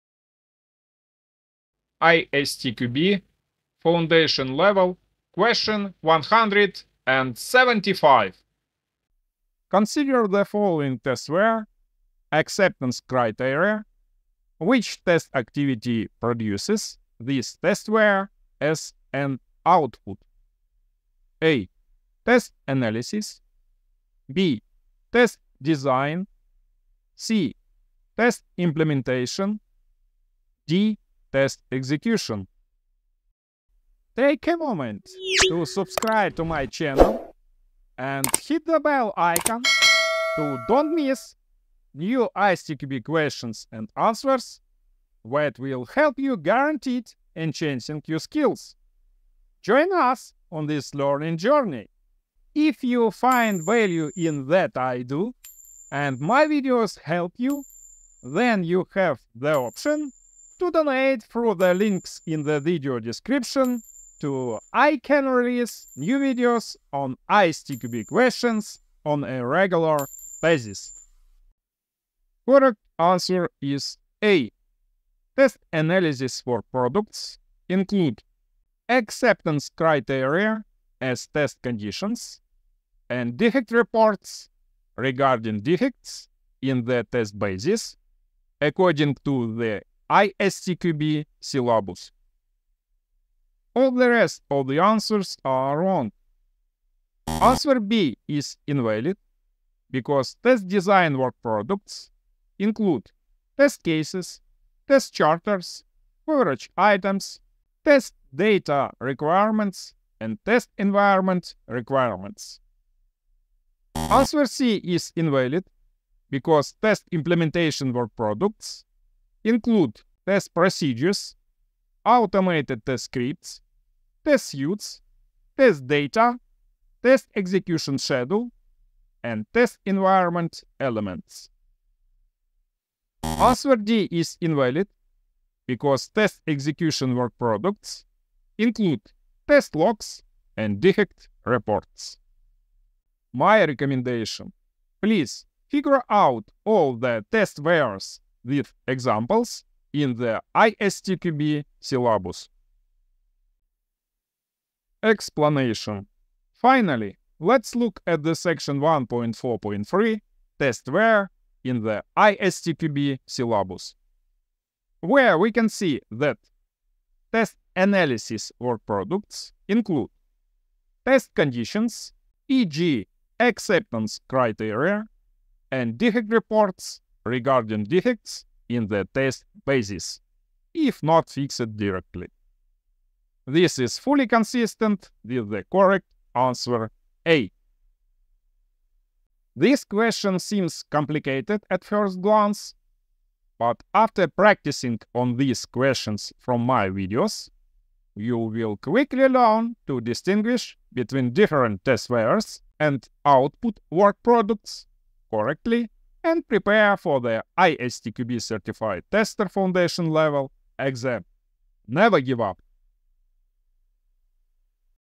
ISTQB. Foundation level v.4.0 (2023). Question #175
ISTQB
Certified Tester Foundation Level v.4.0 (2023)
Question 175.
Consider the following testware: Acceptance criteria.
Which test activity produces this testware as an output?

00:00 Question.
01:11 Answer.
02:33 Explanation.

This question relates to topic in ISTQB Syllabus:
1.4. Test Activities, Testware and Test Roles
1.4.3. Testware

Based on:
1. ISTQB CTFL Syllabus v.4.0